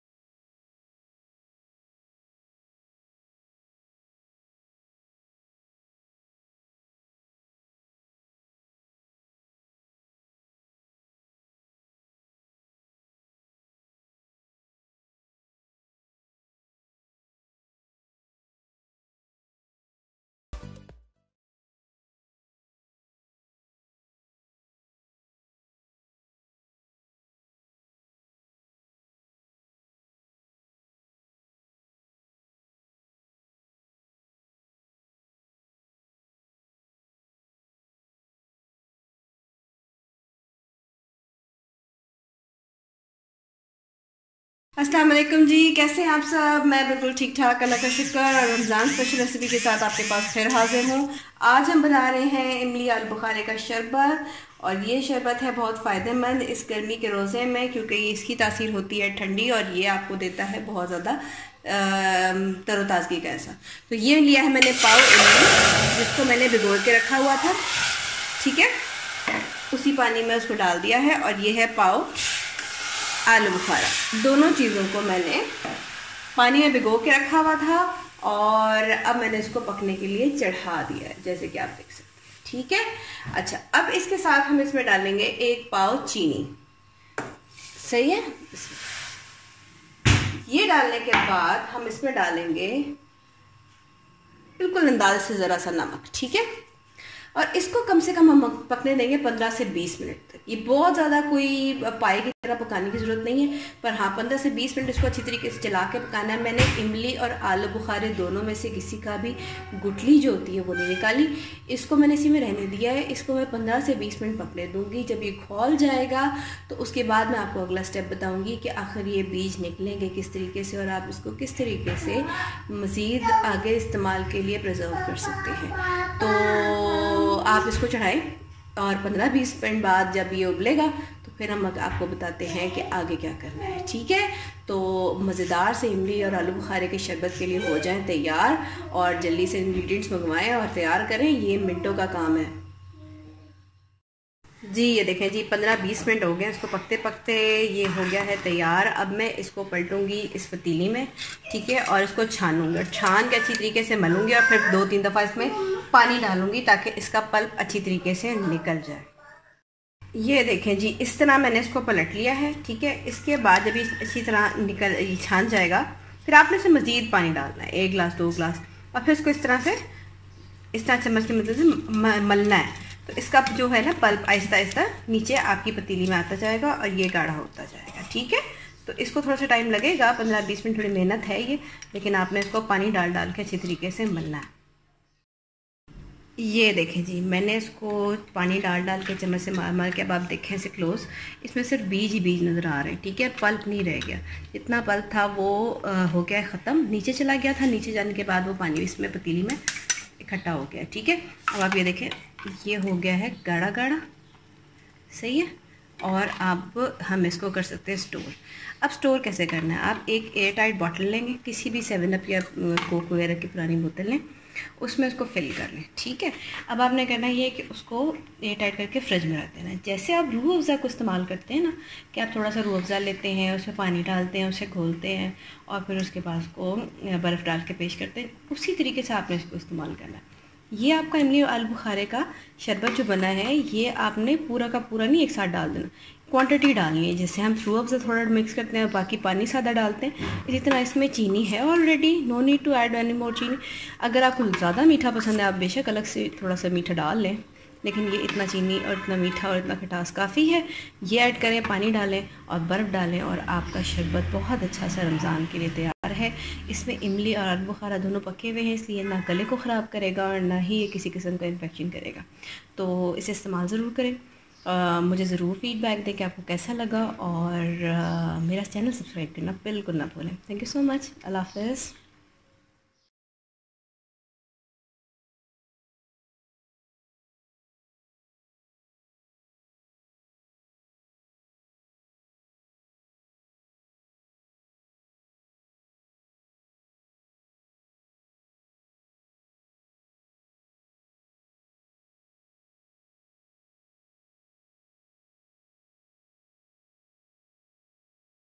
Imli aloo bukharay ka sharbat||ramazan special beverage||plum and tamarind
It is an excellent juice for this summer and specifically for ramazan to stay hydrated and activr. Give it a try you would definitely love the taste